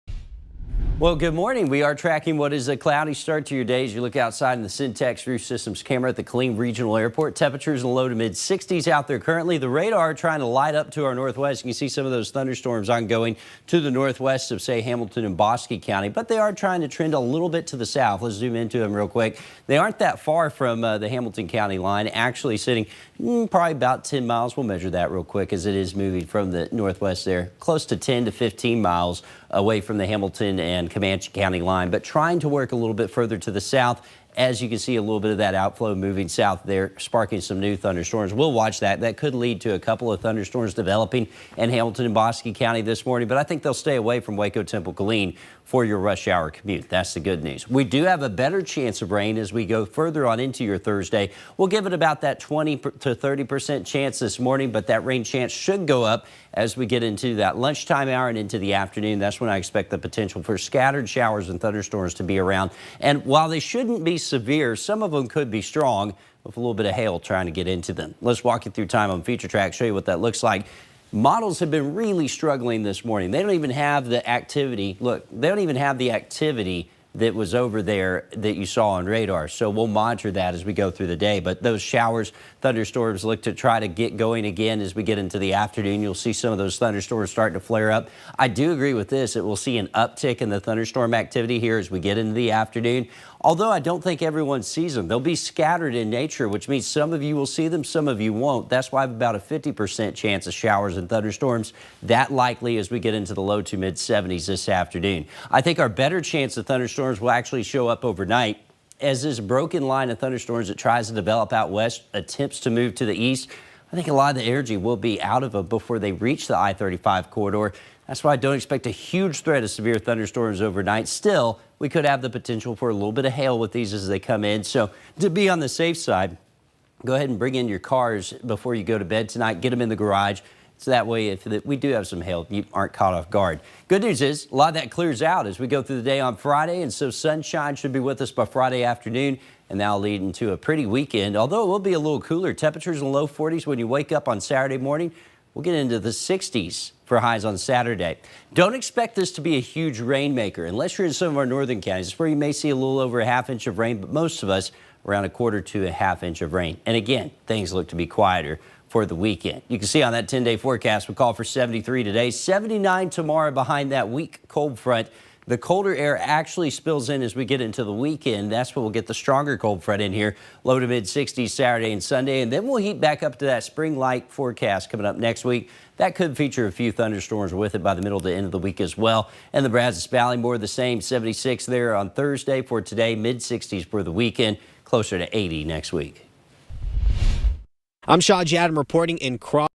Grab the Umbrella! Storms possible Thursday, some strong | 25 Morning Weather
Showers and storms will be possible today as highs climb into the low to mid 70s. Some of these storms could be strong with hail being the main threat. Storm chances clear out by the weekend.